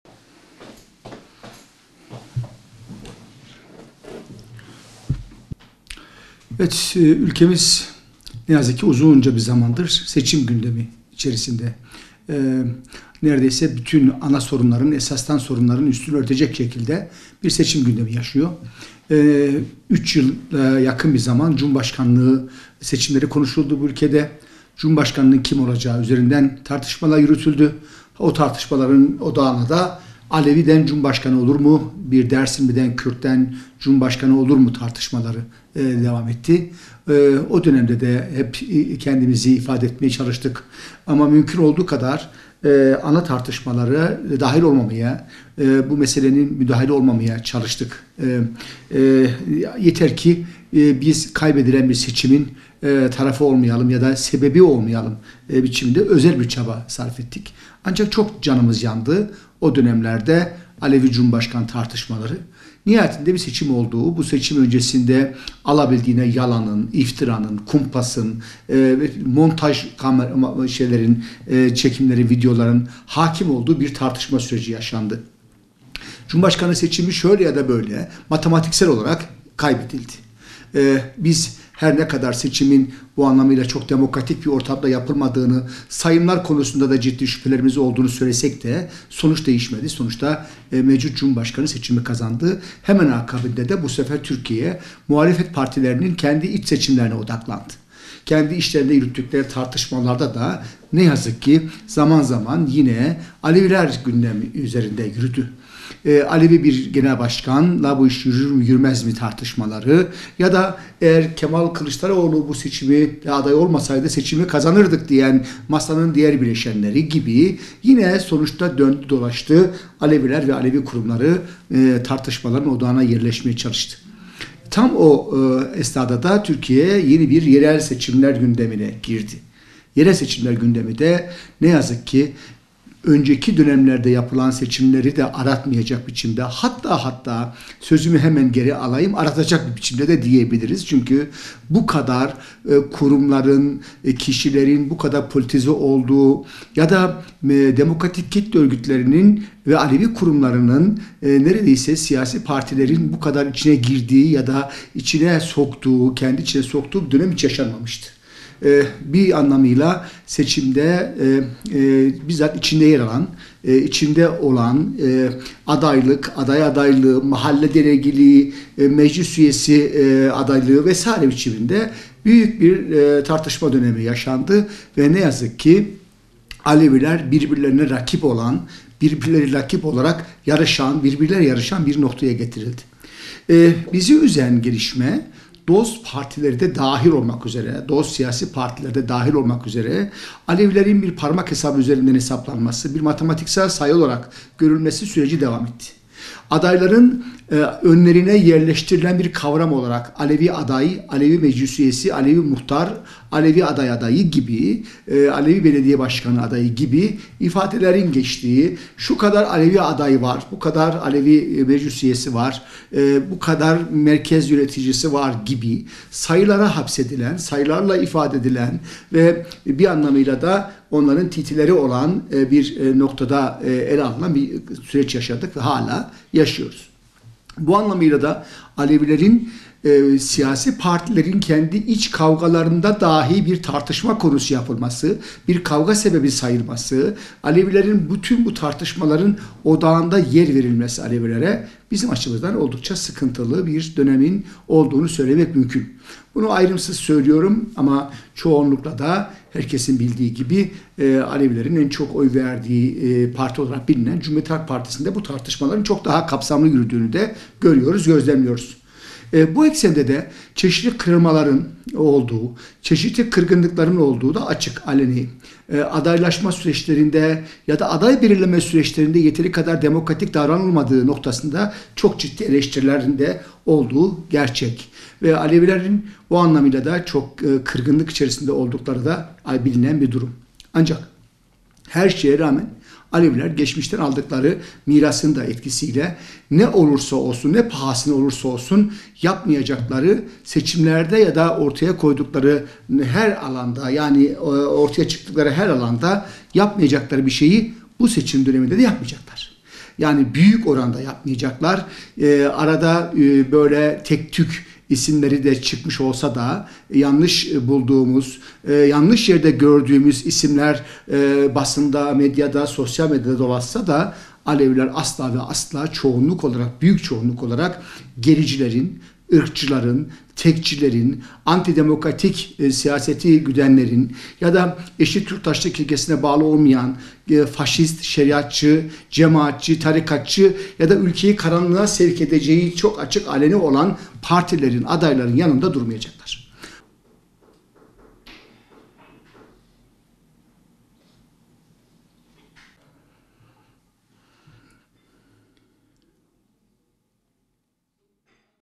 Erçe: Dost partilerin, Alevileri bir parmak hesabı üzerinden görmesi asıl üzücü
PİRHA – PSAKD Genel Başkanı Cuma Erçe, 31 Mart Yerel Seçimlerine gidilirken siyasi partilerin iç tartışmalarında, Alevi kimliğinin kavga sebebi sayılmasının üzücü bir durum olduğunu söyledi. Erçe, “Siyasi partilerin, Alevi kurumlarının bu kadar içine girdiği ya da partileri kendi içine soktuğu bir dönem hiç yaşanmamıştı. Ne yazık ki Aleviler, birbirlerine rakip olan noktaya getirildi” dedi.